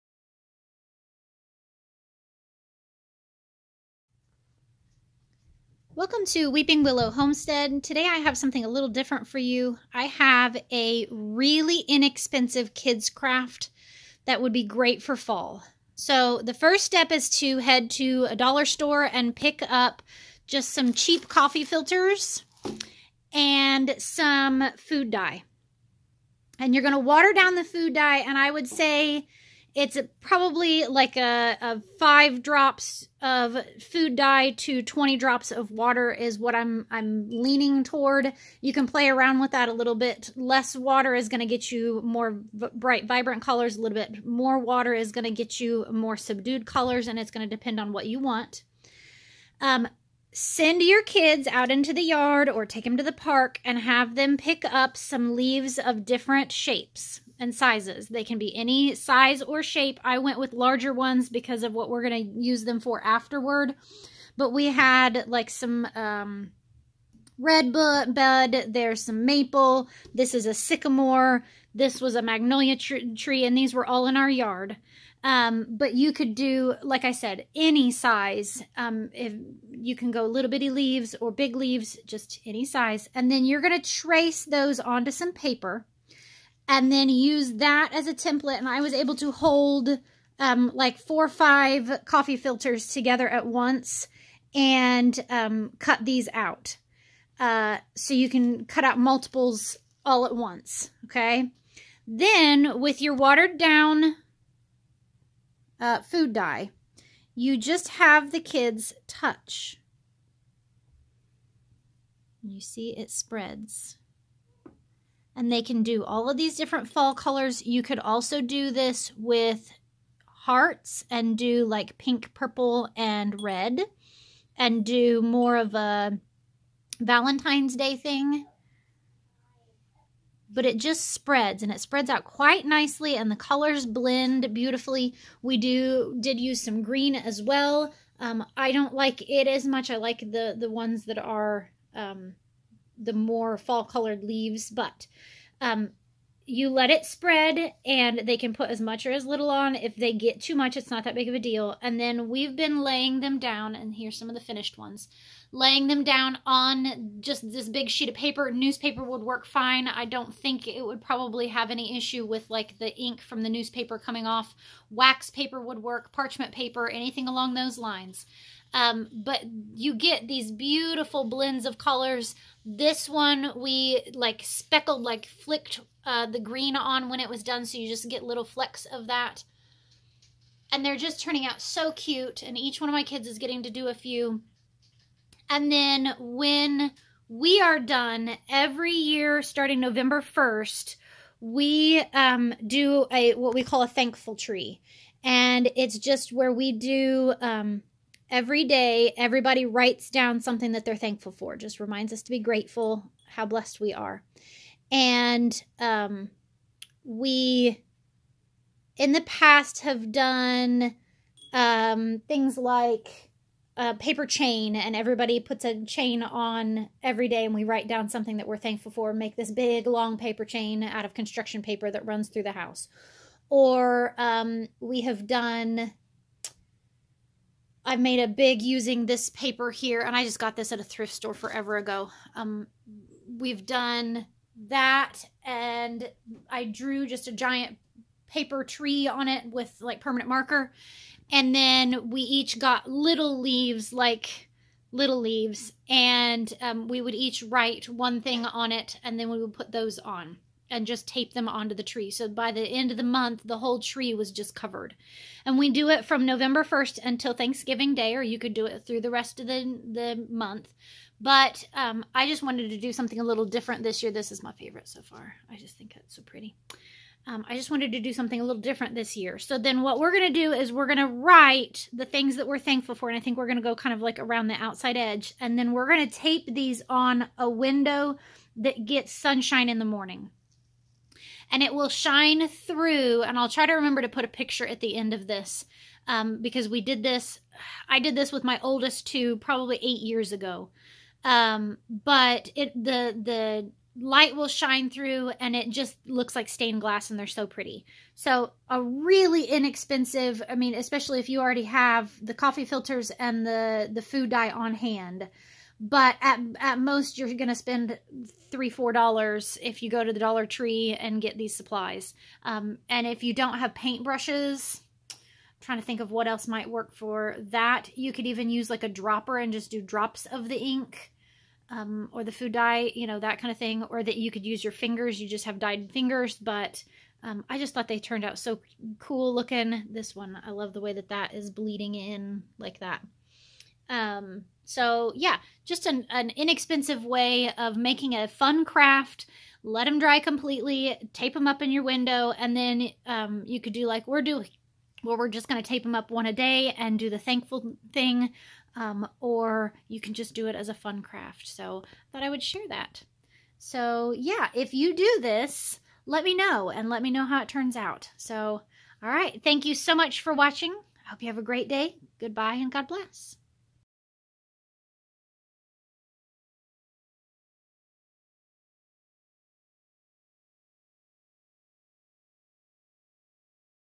Very Inexpensive Fall Kids Craft
Looking for a simple, inexpensive fall craft to do with your kids? Try making these easy coffee filter and food dye leaves.